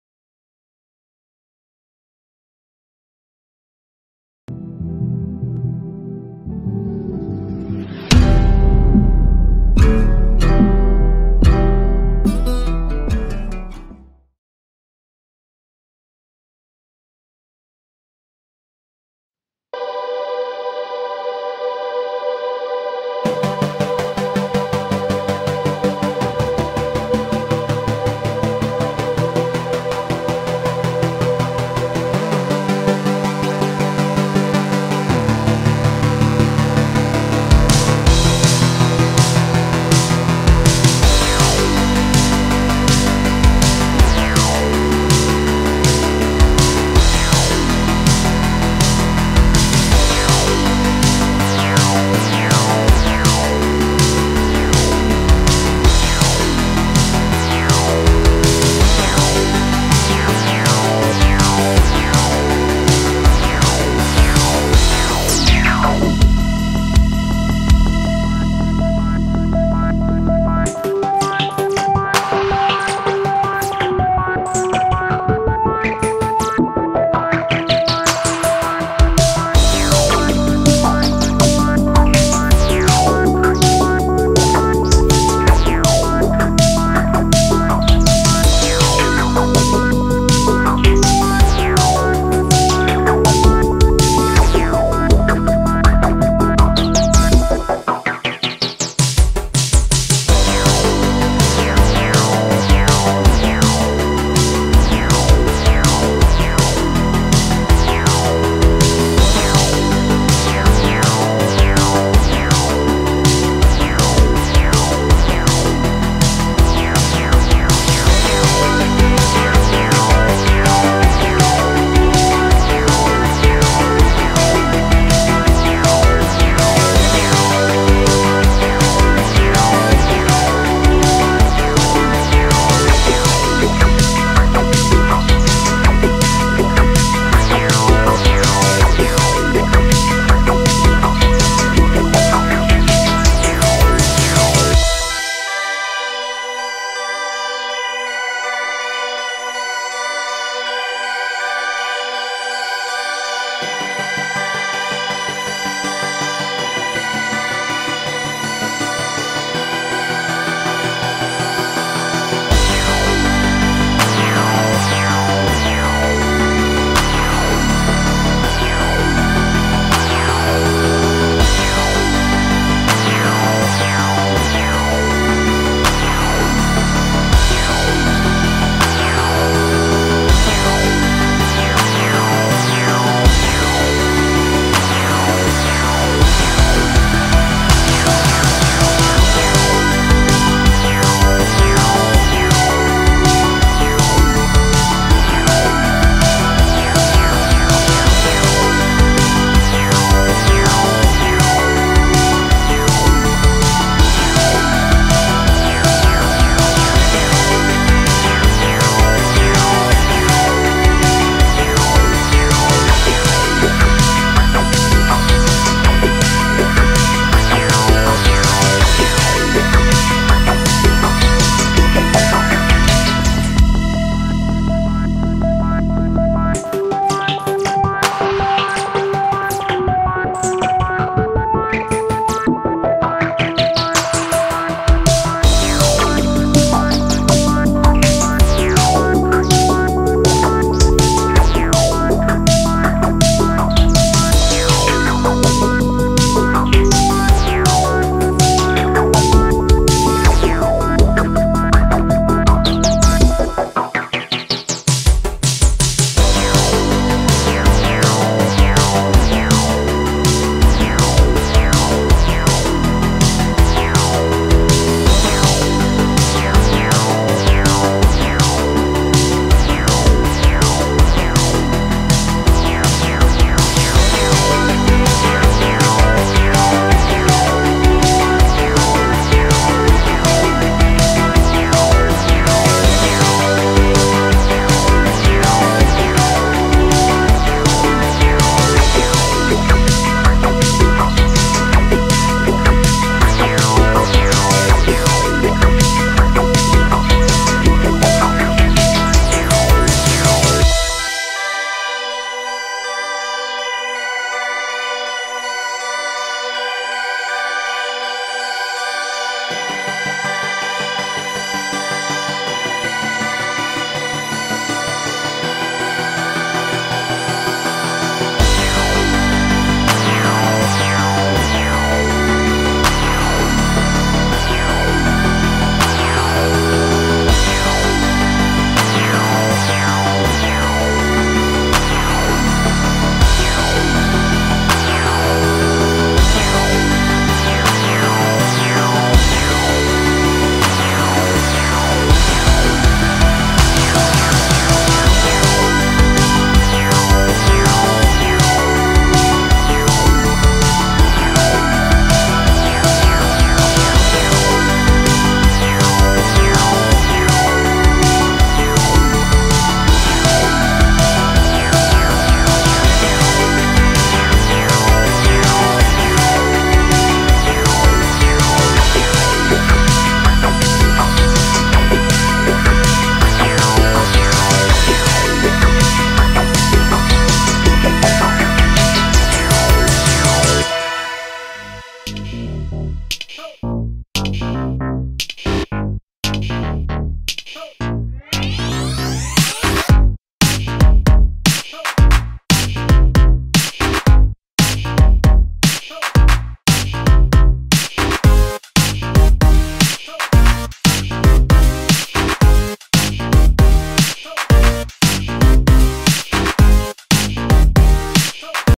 Drawing Overwatch - Ashe New Hero - Animated Short ''Reunion'' Ver Cinematic / Blizzard Heroes
Overwatch Drawing Ashe New Hero character, version Animated Short ''Reunion'' Ver Cinematic / Blizzard Heroes Drawing 3d realistic, dibujando a Ashe nuevo personaje de overwatch

More videos / Mas videos!